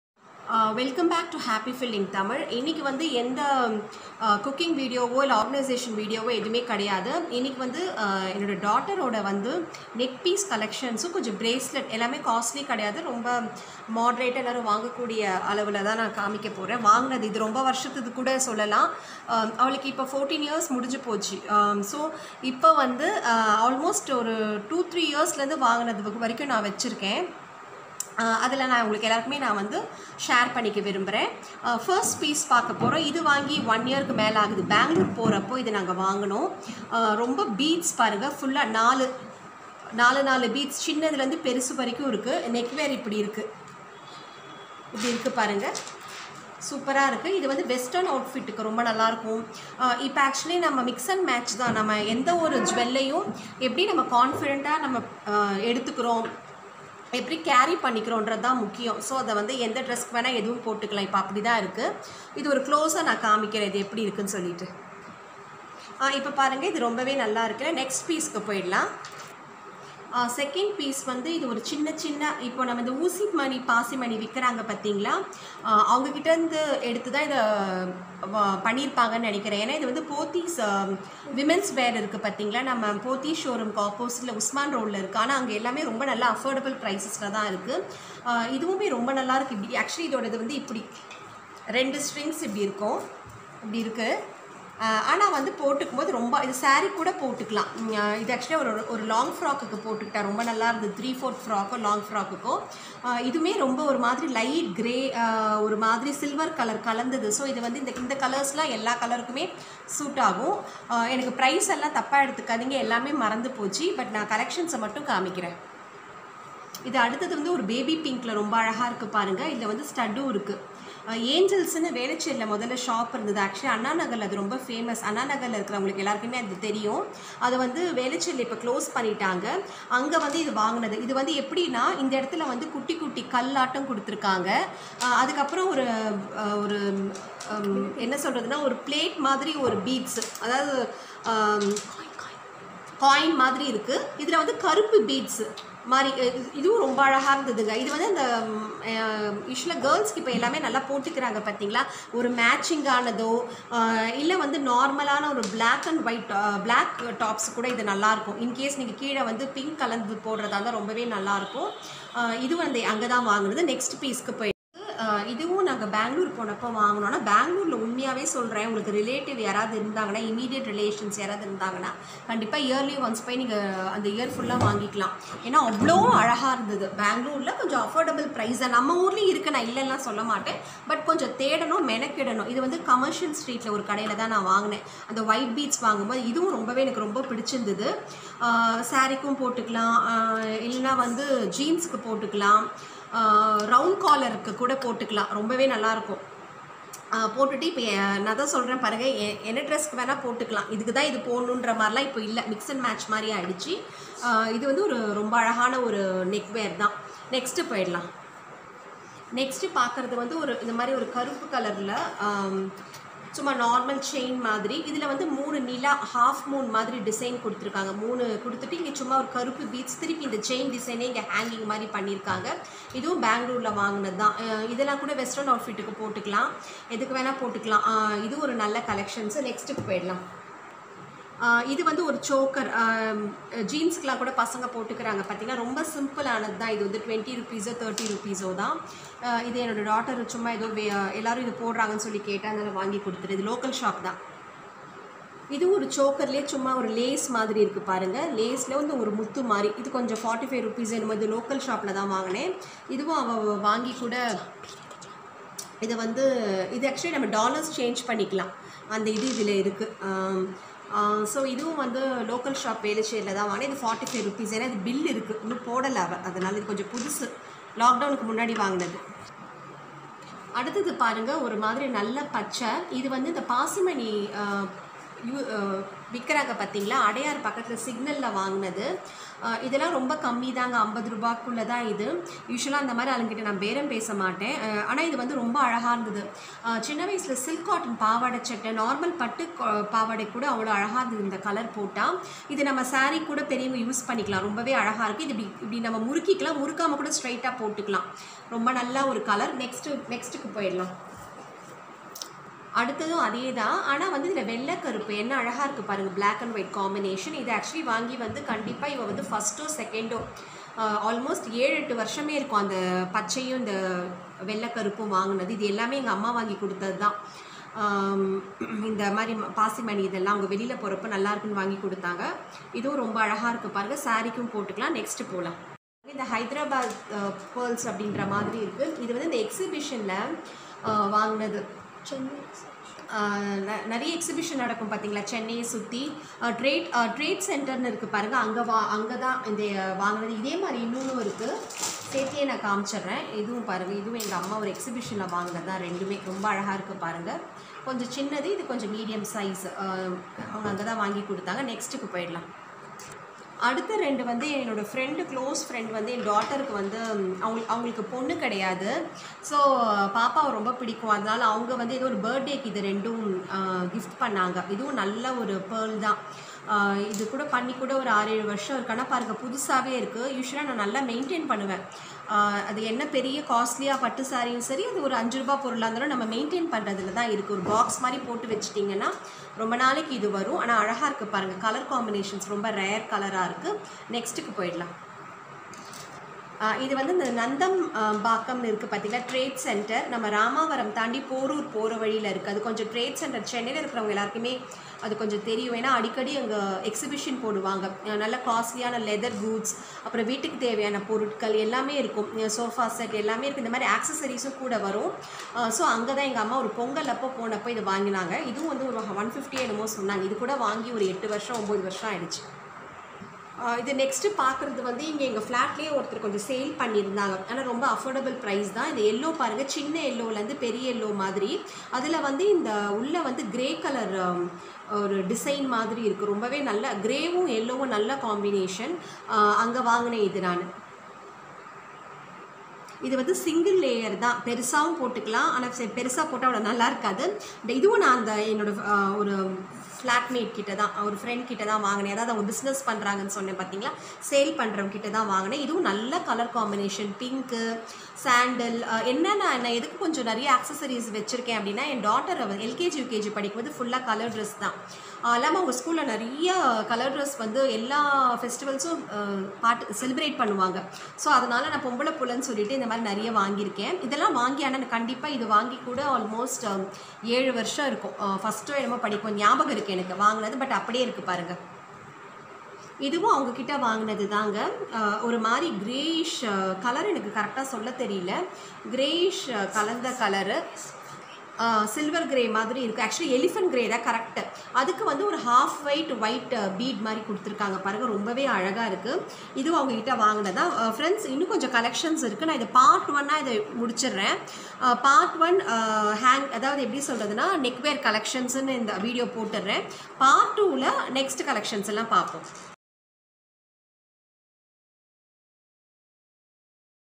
கழுத்து அணிகலன்கள் - 1 | Neck wear Collections - Part 1
I have shown my daughters neckwear collections. kindly watch it fully